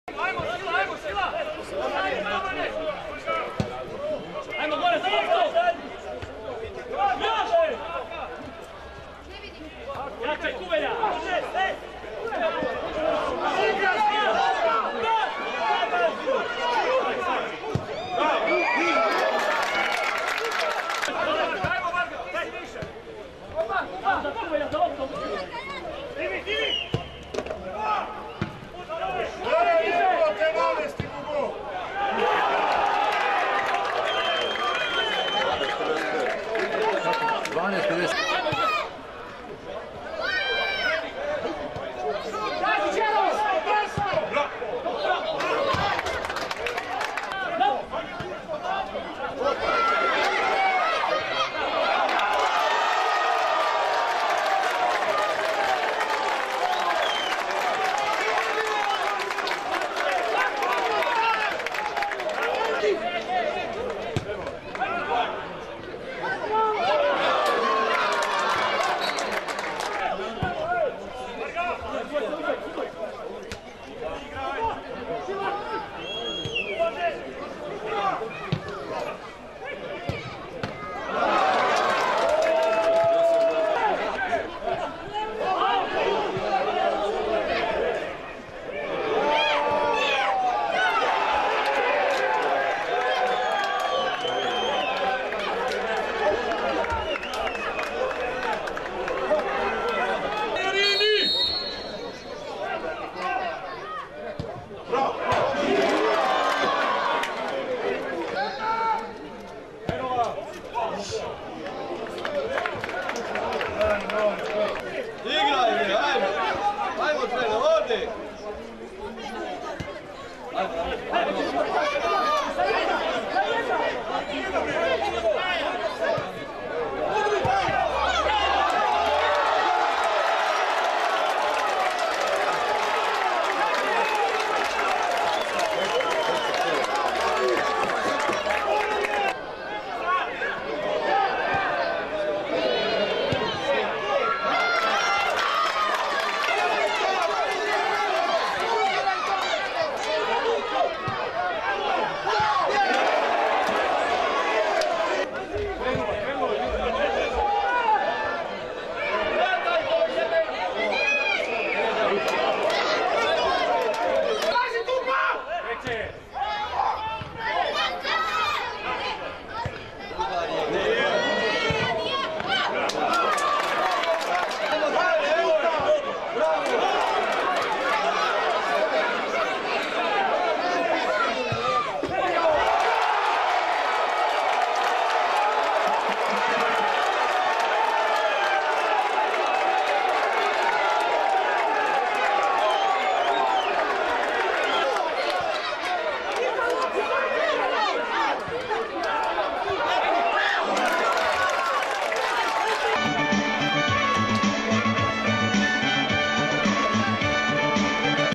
Linglong Tire Super liga 2019/20 - 12.Kolo: JAVOR MATIS – TSC 3:2 (1:2)
Linglong Tire Super liga Srbije - 12.Kolo: JAVOR MATIS – TSC 3:2 (1:2)
Strelci: Alija Krnić 23', Nikola Kuveljić 60', Miroslav Maričić 87' - Srđan Grabež 11', Dejan Milićević 41'